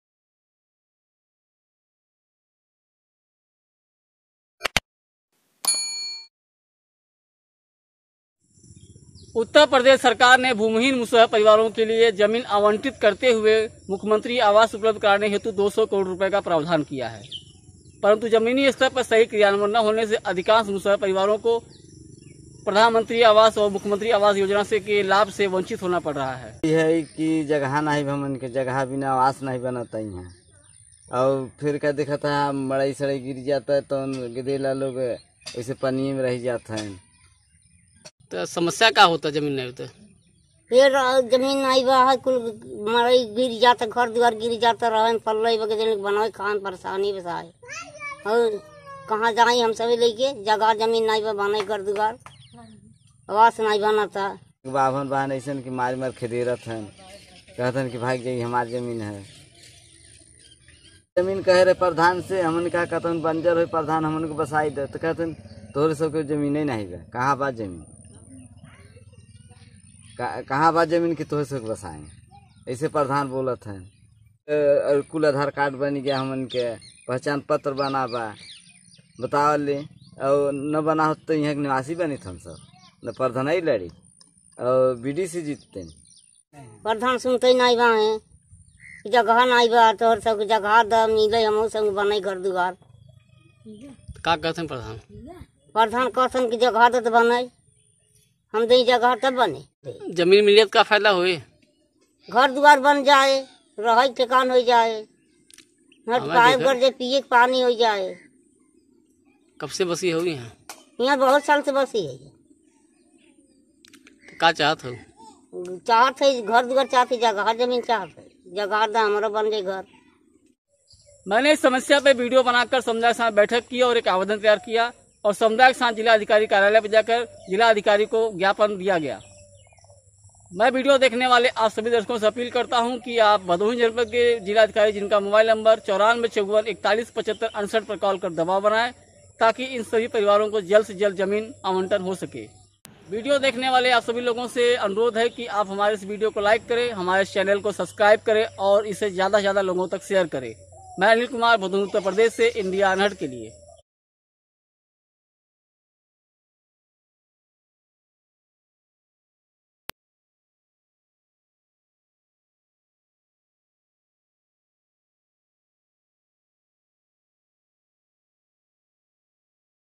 भूमिहीन मुसहर समुदाय को आवास योजना का लाभ नहीं
मर्चवार सनवईया गाँव, जिला भदोई ,उत्त्तर प्रदेश जहा  मुसहर परिवार काफी संख्या में रहते है, महादलित समुदाय से आने वाला यह समुदाय अभी भी समाज के सब से निचले पायदान पर है. काफी समय से सामाजिक  और आर्थिक  विसमता का सामना कर रहा यह समुदाय भूमिहीन है. और इस कारण प्रधानमंत्री आवास योजना के तहत मिलने वाला आवासीय पट्टा न होने से 12 मुसहर परिवारो को प्रधानमंत्री आवास योजना व मुख्यमंत्री आवास योजना के लाभ से वंचित होना पड़ रहा है ।

Community Correspondent Anil Kumar reports from Bhadoi, Uttar Pradesh  for Video Volunteers.